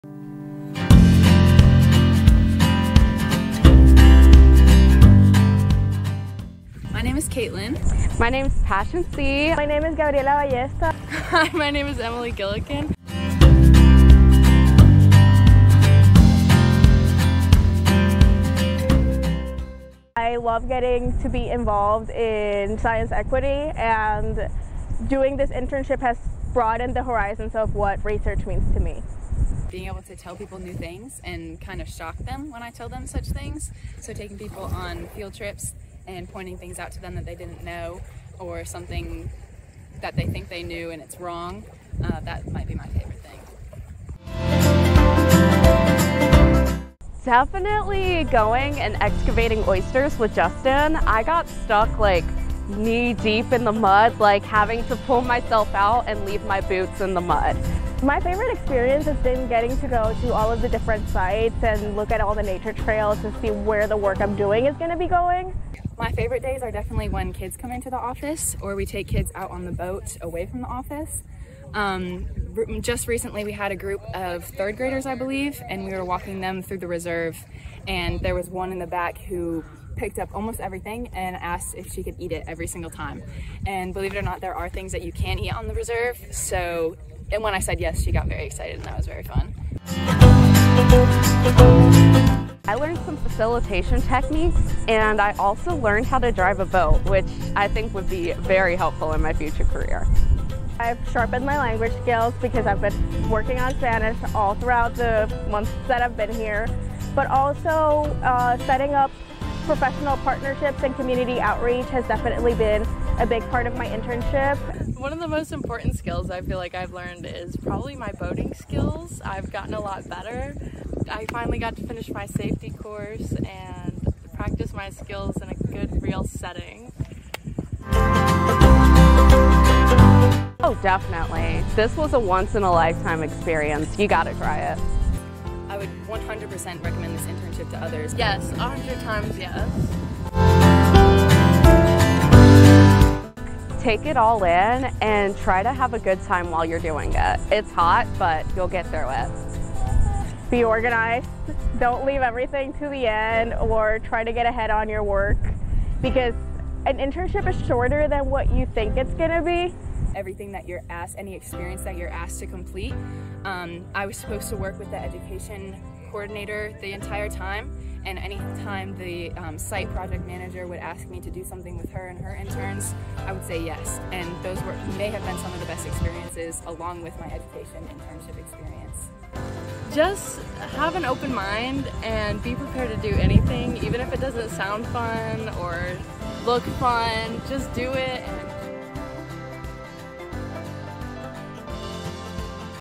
2023 Summer Interns - NC Coastal Reserve
Learn about the 2023 summer interns' experiences with the North Carolina Coastal Reserve!